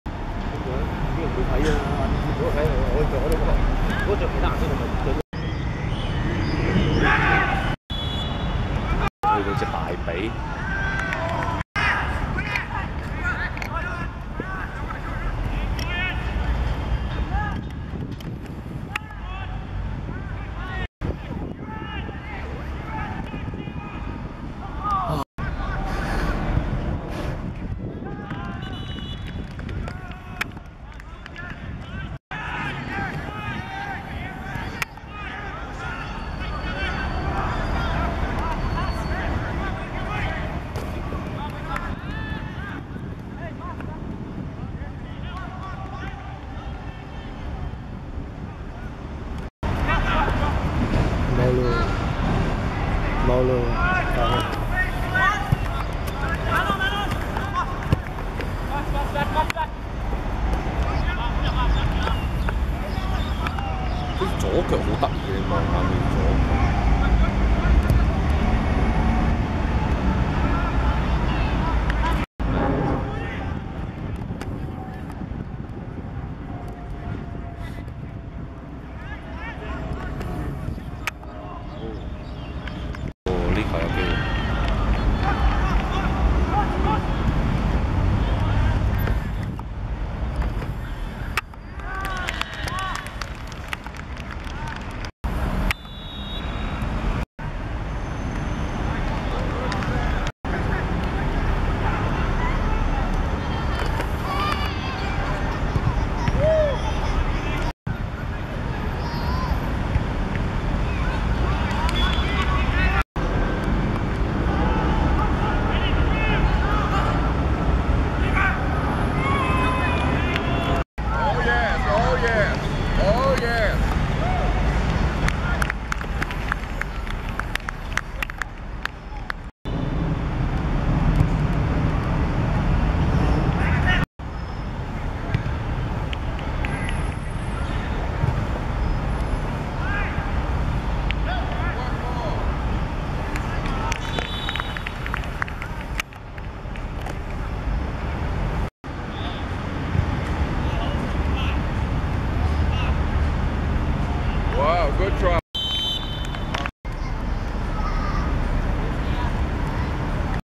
香港國際vs英皇佐治(2015.11.3.D1學界足球甲組)精華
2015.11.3.高士威球塲 16:20 香港國際(紅) 4 : 0 英皇佐治(黑)